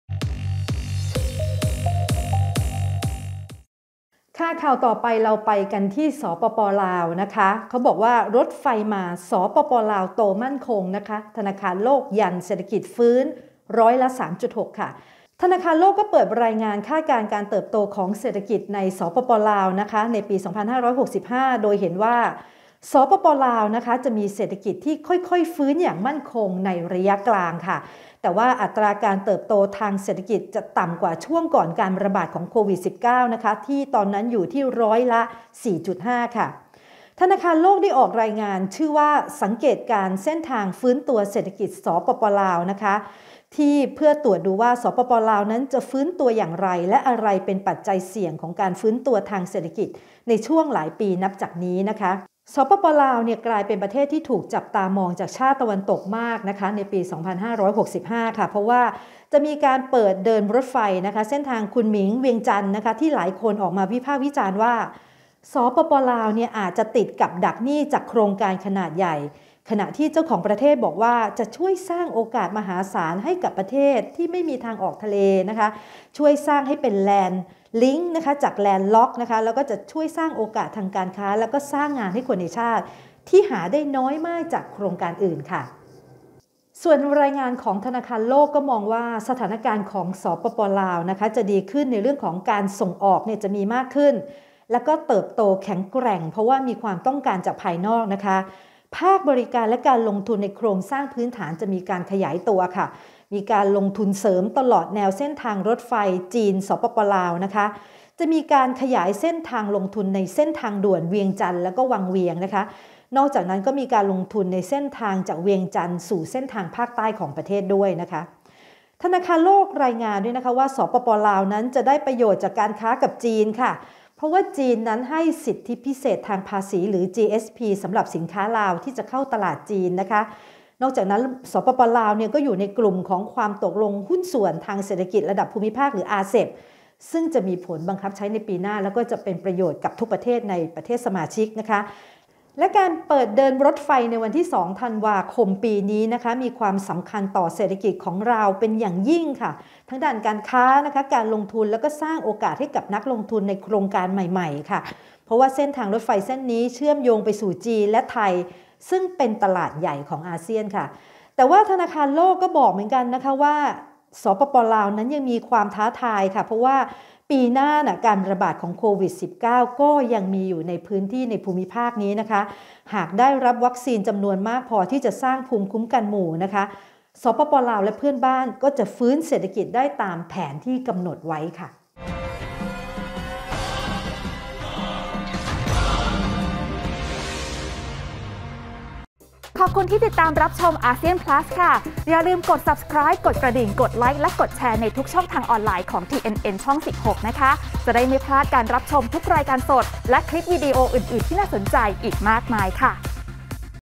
รถไฟมาสปป.ลาวโตมั่นคง ธนาคารโลกยันเศรษฐกิจฟื้นร้อยละ 3.6
ธนาคารโลกเปิดรายงาน คาดการณ์การเติบโตของเศรษฐกิจ สปป.ลาวในปี 2565 โดยเห็นว่า เศรษฐกิจของสปป.ลาว จะค่อยๆฟื้นอย่างมั่นคงในระยะกลาง แต่อัตราการเติบโตของเศรษฐกิจจะต่ำกว่าช่วงก่อนการระบาดโควิด19 คืออยู่ที่ร้อยละ3.6  จากที่คาดเอาไว้ที่ร้อยละ 4.5

ธนาคารโลกได้ออกรายงานชื่อว่า "สังเกตการณ์เส้นทางฟื้นตัวเศรษฐกิจสปป.ลาว" เพื่อตรวจดูว่า สปป.ลาวจะฟื้นตัวอย่างไร และอะไรเป็นปัจจัยเสี่ยงของการฟื้นตัวทางเศรษฐกิจในช่วงหล่ายปีนับจากนี้

สปป.ลาวกลายเป็นประเทศที่ถูกจับตามองจากชาติตะวันตกมาก เป็นพิเศษในปี 2565  เพราะจะมีการเดินรถไฟเส้นทาง คุนหมิง-เวียงจันทน์ ด้วยเทคโนโลยีรถไฟความเร็วสูงแต่วิ่งด้วยระดับความเร็วปานกลาง ที่หลายคนออกมาวิพากษ์วิจารณ์ว่า สปป.ลาวอาจจะติดกับดักหนี้จากโครงการขนาดใหญ่ ขณะที่เจ้าของประเทศบอกว่า จะช่วยสร้างโอกาสมหาศาลให้กับประเทศที่ไม่มีทางออกทะเล ช่วยสร้างแลนด์ล็อค เป็นแลนด์ลิงค์ และช่วยสร้างโอกาสทางการค้าและสร้างงานให้คนในชาติ ทีหาได้น้อยจากโครงการอื่น

ส่วนรายงานจากธนาคารโลกมองว่า สถานการณ์ของสปป.ลาวจะดีขึ้น
- การส่งออกมากขึ้น และเติบโตแข็งแกร่ง เพราะมีความต้องการ จากภายนอก
- ภาคบริการและการลงทุนในโครงสร้างพื้นฐานจะขยายตัว
- มีการลงทุนเสริม ตลอดแนวเส้นทางรถไฟจีน-สปป.ลาว
- ขยายการลงทุนเส้นทางด่วนเวียงจันทน์-วังเวียง
- การลงทุนในเส้นทางจากเวียงจันทน์ สู่เส้นทางภาคใต้

ธนาคารโลก รายงานว่า สปป.ลาวจะได้ประโยชน์ จากการค้ากับจีน เพราะจีนให้สิทธิพิเศษทางภาษี หรือ GSP สำหรับสินค้าลาว ที่จะเข้าตลาดจีน  นอกจากนั้นสปป.ลาว ร่วมอยู่ใน ความตกลงหุ้นส่วนทางเศรษฐกิจระดับภูมิภาค RCEP ซึ่งมีผลบังคับใช้ปี 2565

และการเปิดเดินรถไฟในวันที่ 2 ธันวาคม ปีนี้ มีความสำคัญต่อเศรษฐกิจลาวอย่างยิ่ง ทั้งการค้า การลงทุน และสร้างโอกาสให้นักลงทุน ในโครงการใหม่ๆ เพราะเส้นทางรถไฟเส้นนี้ เชื่อมโยงไปสู่จีน และไทย ตลาดขนาดใหญ่ของอาเซียน

อย่างไรก็ดี ความท้าทายของสปป.ลาว ในปีหน้านี้คือการระบาดของโควิด19 ที่ยังครอบคลุมทั้งภูมิภาค หากได้วัคซีนจำนวนมากพอที่จะสร้างภูมิคุ้มกันหมู่ สปป.ลาวและเพื่อนบ้าน ก็จะฟื้นเศรษฐกิจได้ตามแผนที่กำหนดไว้